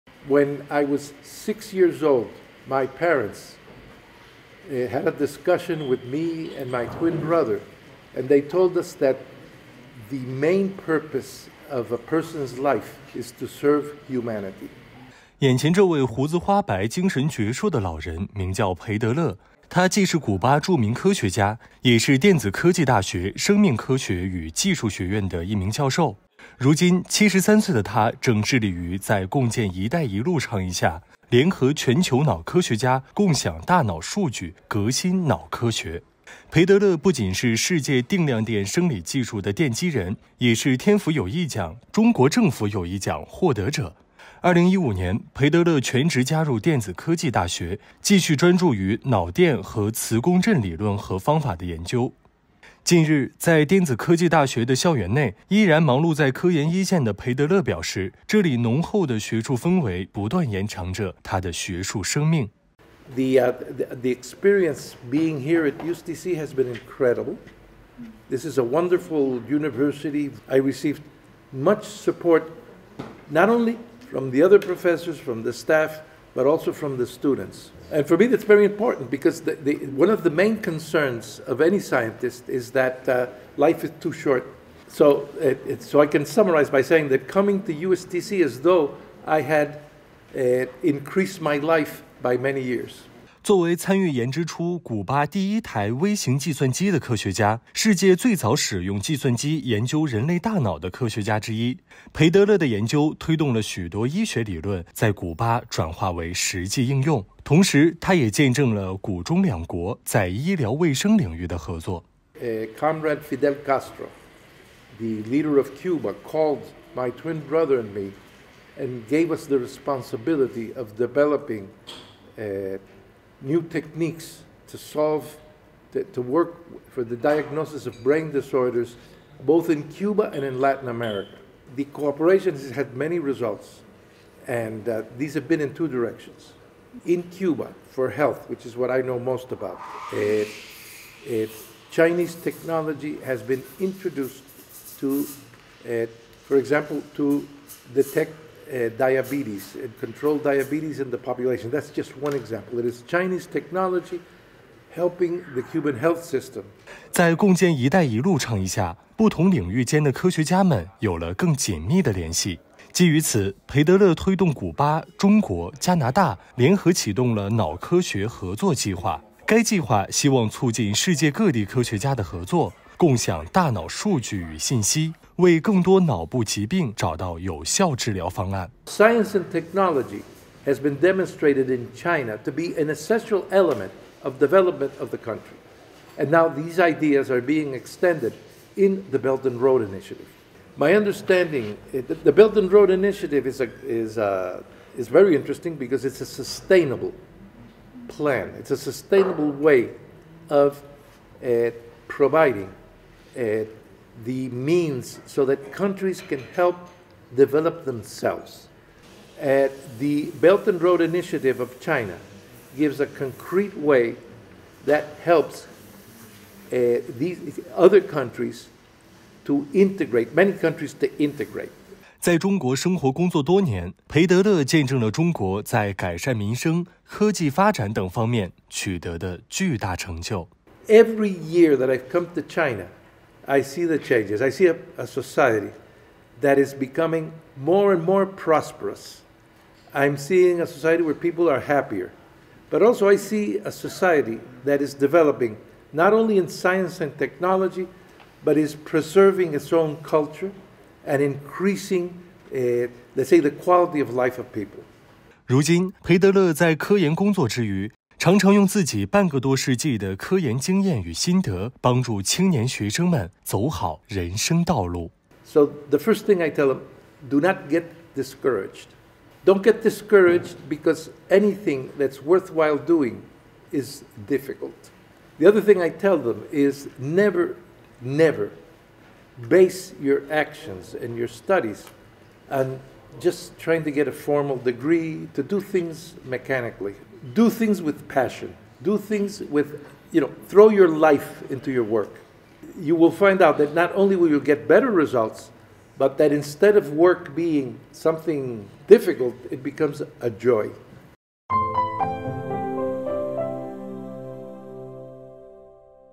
古巴著名科学家裴德乐：来到中国，我的学术生命仿佛延长
眼前这位胡子花白、精神矍铄的老人名叫裴德乐，他既是古巴著名科学家，也是电子科技大学生命科学与技术学院的一名教授。如今，73岁的他正致力于在共建“一带一路”倡议下，联合全球脑科学家，共享大脑数据，革新脑科学。

裴德乐不仅是世界定量电生理技术的奠基人，也是“天府友谊奖”“中国政府友谊奖”获得者。2015年，裴德乐全职加入电子科技大学，继续专注于脑电和磁共振理论和方法的研究。近日，在电子科技大学的校园内，依然忙碌在科研一线的裴德乐表示，这里浓厚的学术氛围不断延长着他的学术生命。